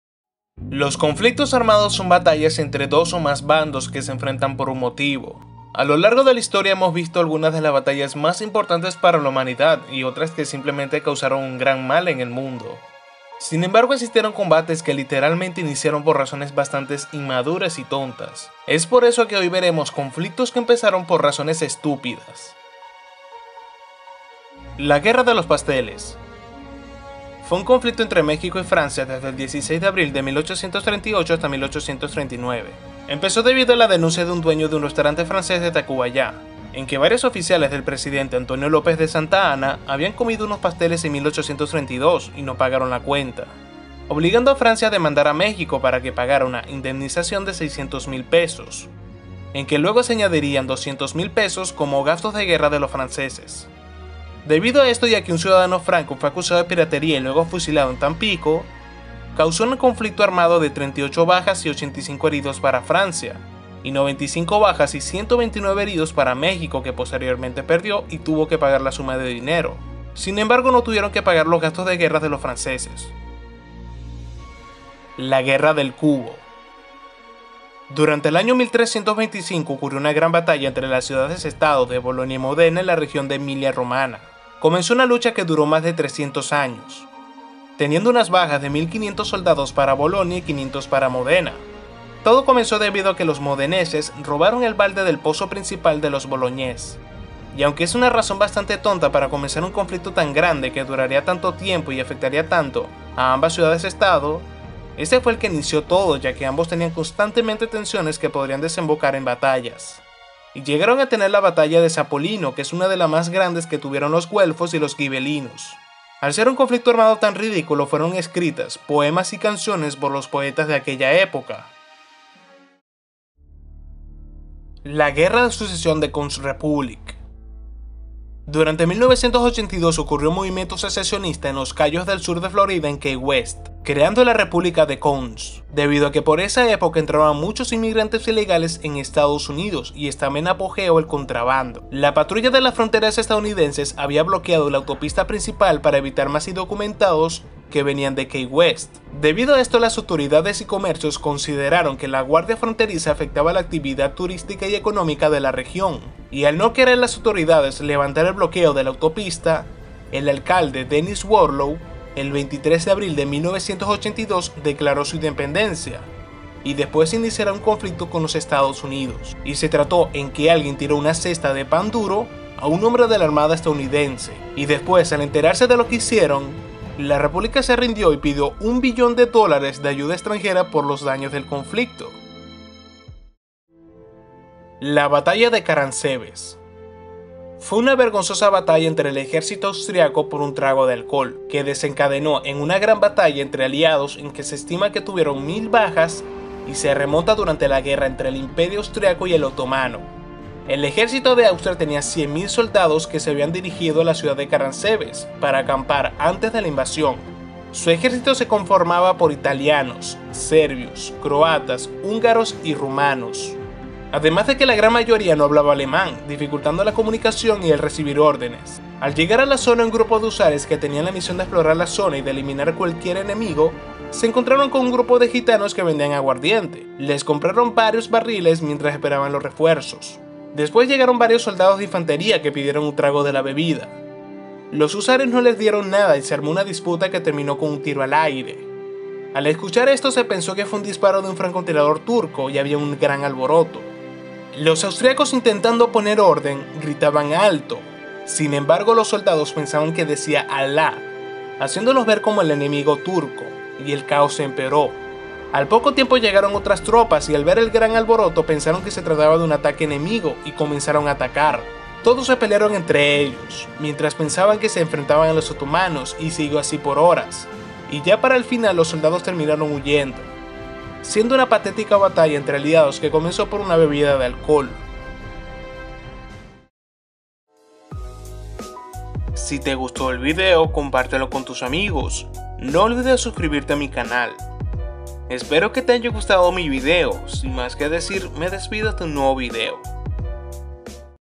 Conflictos que Empezaron por Razones Estupidas
Durante toda la historia han existido batallas importantes que han cambiado a la historia pero ¿conoces sobre estas? ¿Sabias que florida se independizo de los estados unidos? ¿Qué comenzó un conflicto armado por unos pasteles? Si quieres saber mas quedate en el video...